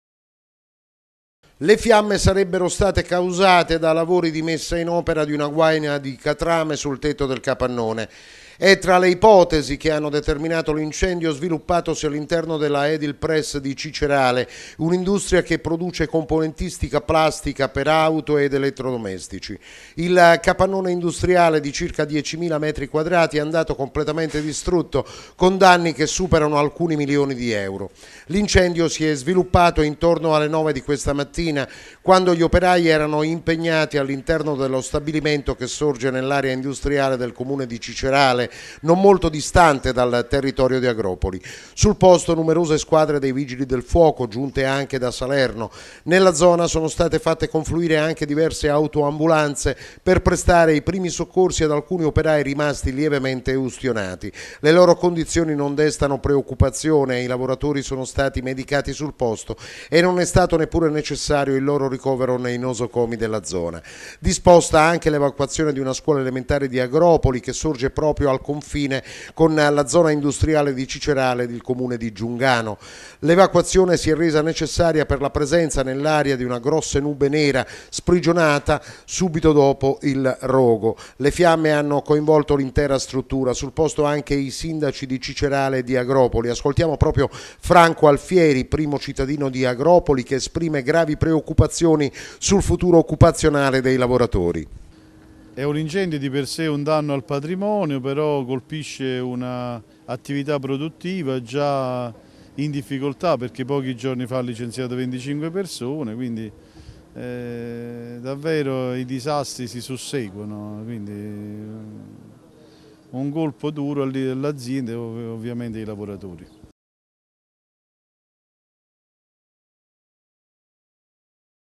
TG 040412 Fabbrica in fiamme a Cicerale. Evacuata una scuola
Un vasto incendio ha distrutto questa mattina a Cicerale, nel Cilento, uno stabilimento che produce componentistica plastica per auto ed elettrodomestici. Alcuni operai rimasti lievemente ustionati. Evacuata una scuola elementare.